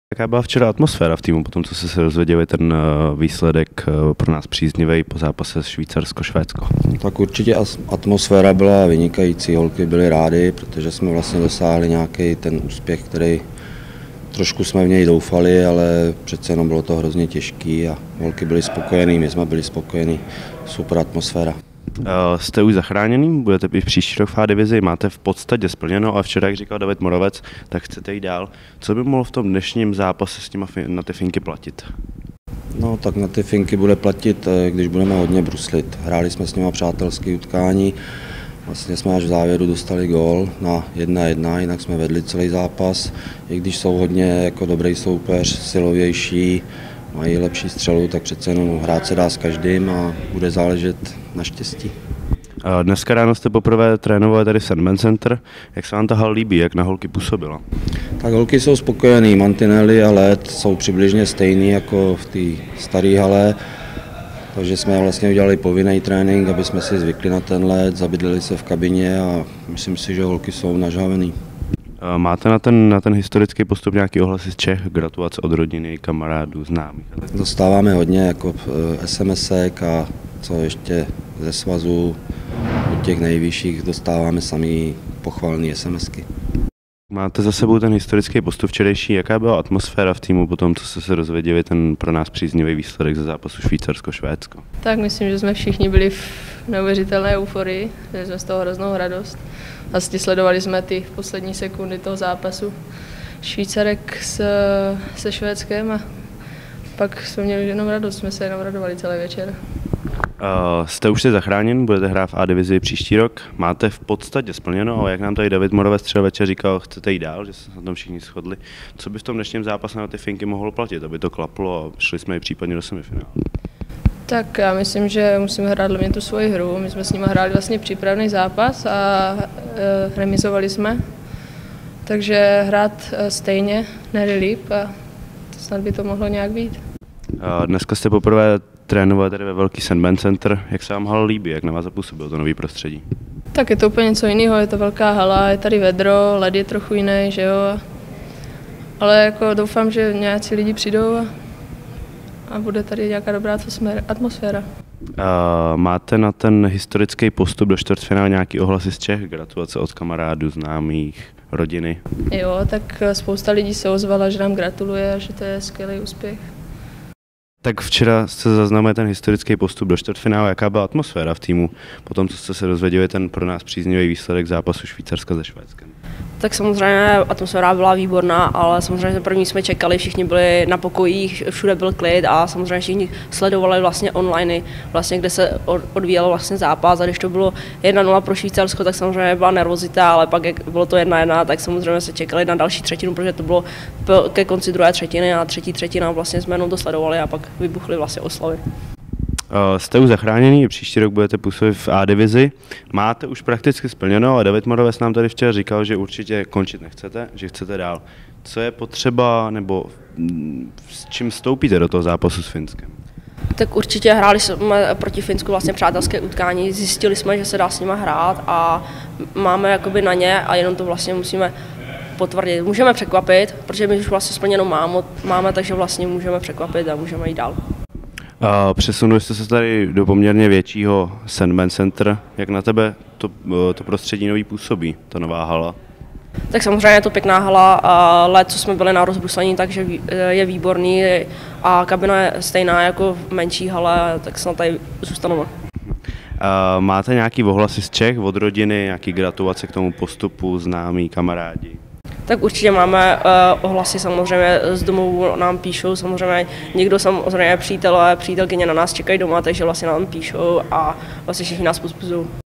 Rozhovory před čtvrtfinále proti Finsku: Vozák, Přibylová a Studentová
Trenér Jiří Vozák, útočníce Vendula Přibylová a Simona Studentová vyhlížejí čtvrtfinálovou bitvu, v níž se česká reprezentace žen střetne se silným Finskem.